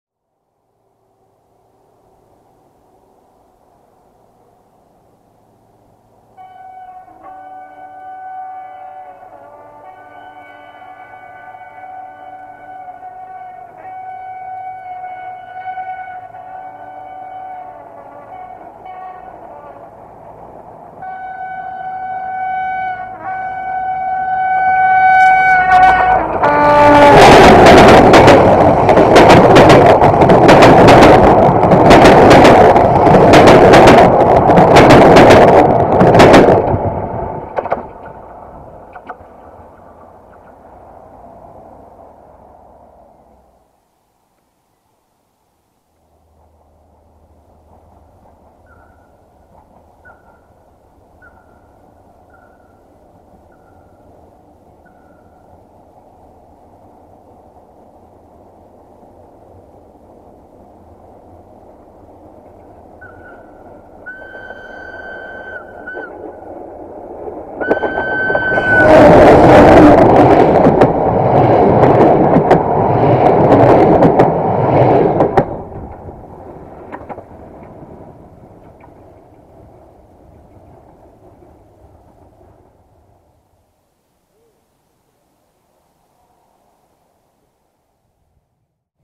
Sub/Under GM 65-1300-6&Fluieroasa/Whistle Loko 60-0858-0 in Muntii Apuseni Mountains - 02 June 2017
1:IR367-2"Harghita" - Budapesta Keleti - Brasov/Targu Mures - Regina Bihorului~Jimmy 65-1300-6 RO-SNTFC
2:R3076 - Oradea - Cluj Napoca - 060-DA/LDE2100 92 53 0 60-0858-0 RO-SNTFC

02 June 2017